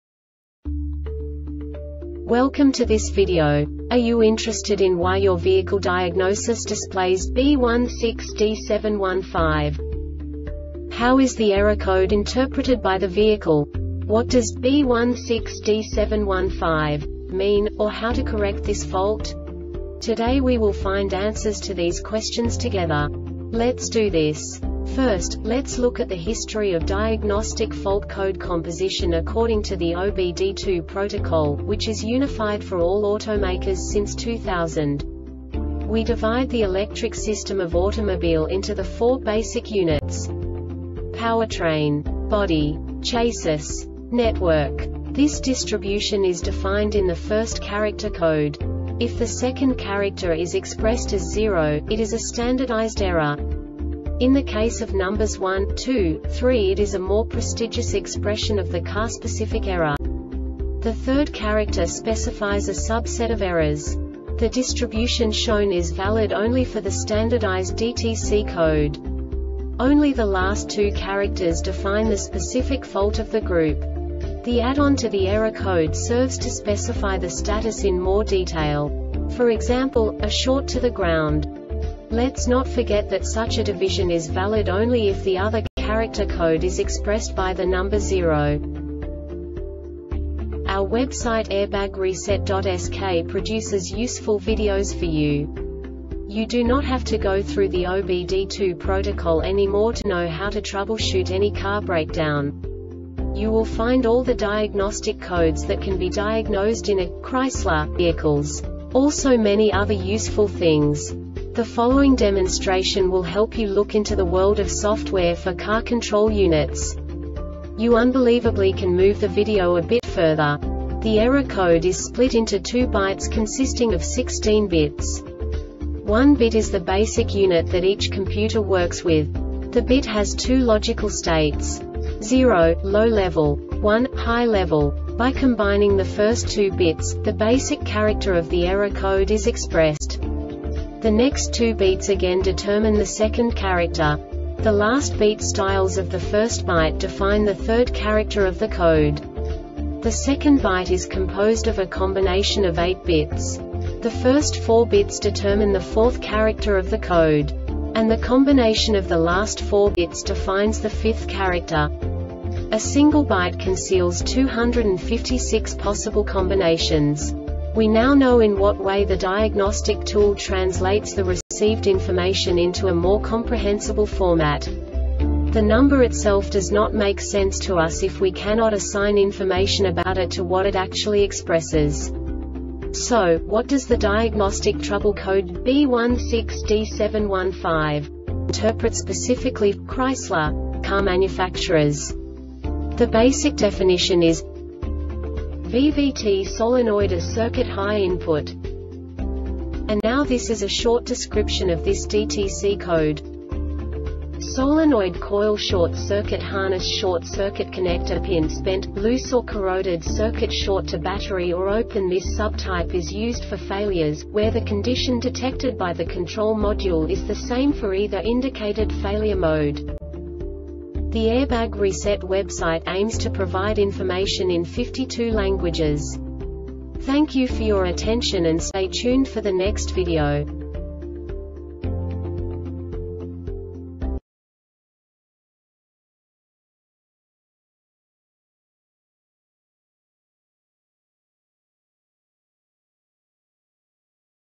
DTC Chrysler B16D7-15 Short Explanation
Contents:
0:21 Basic DTC analysis according to OBD2 protocol standard.
1:48 Insight into programming
2:58 A diagnostically understandable description of the Diagnostic Trouble Code
3:05 Basic error definition

Make:
Chrysler
Code:
B16D7 15
Definition:
VVT solenoid A circuit high input
Cause:
Solenoid coil short circuitHarness short circuitConnector pins(s) bent, loose or corroded
Failure Type: